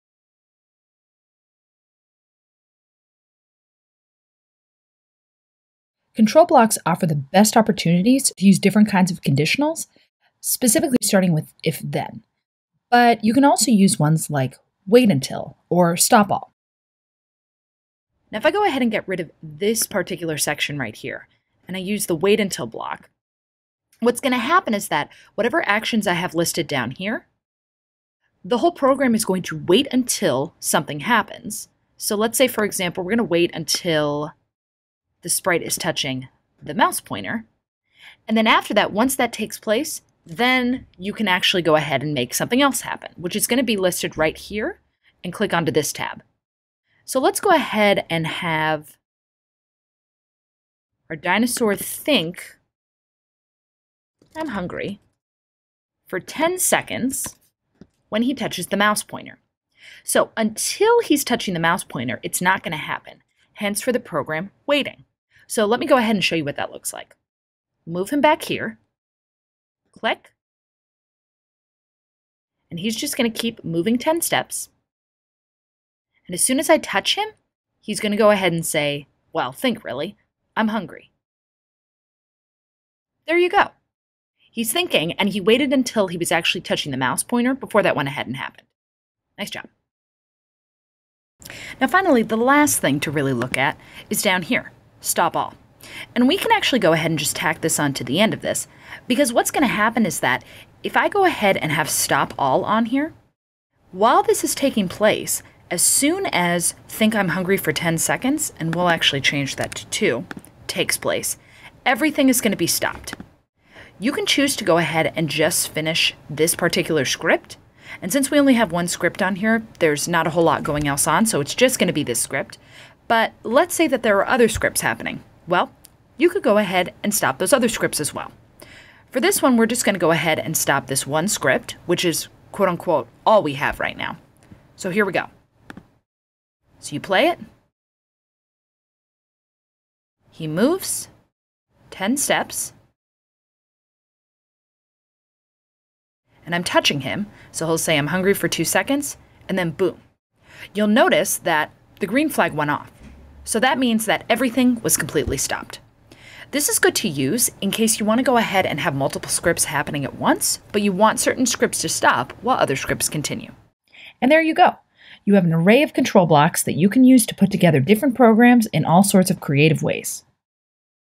Control: Part 1
An introduction to control blocks in Scratch.